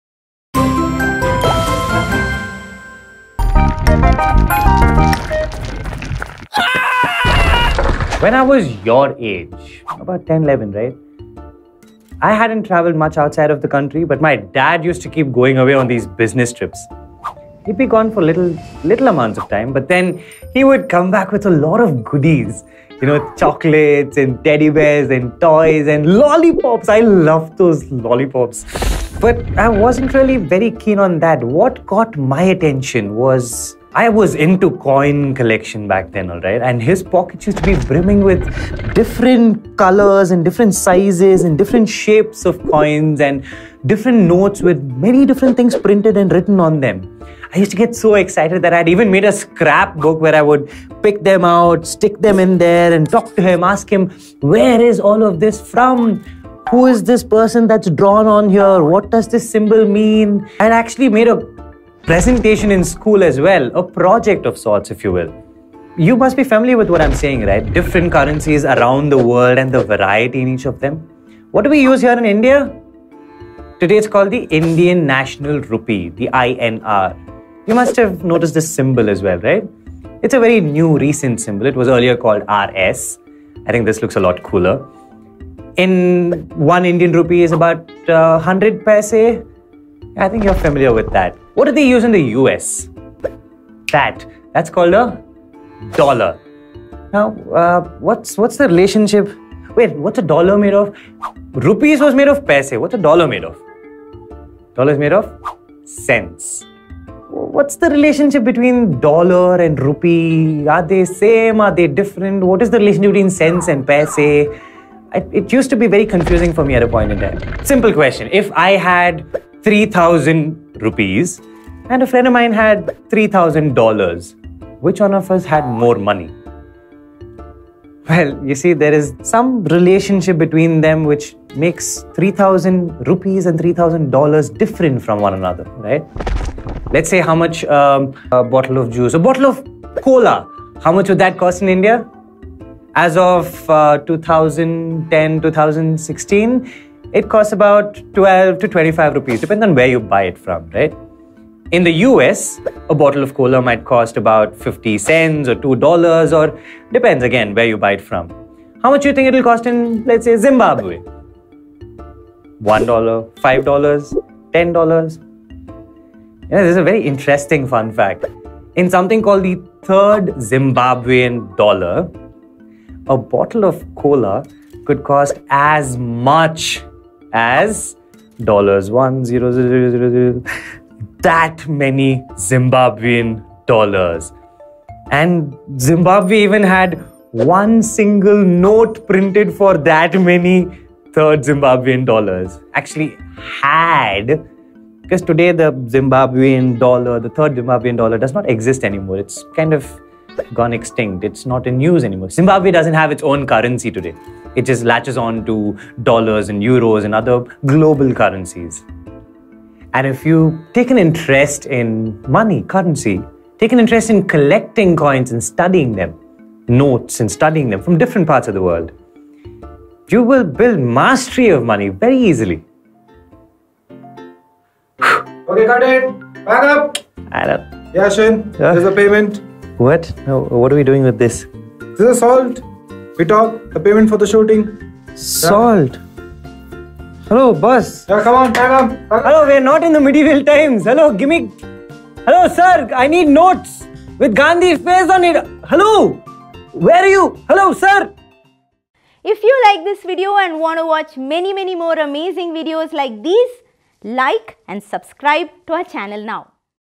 About Currency | Class 4 I Learn with BYJU'S
Did you know that there are about 180 currencies in the world? Did you know that every currency has a unique symbol?

Do you know what the Indian currency’s symbol is? Watch this video on 'Currency' to find the answers to questions like these and more. Discover the new revolutionary way of learning with India's most loved math and science learning program. Join the learning revolution today!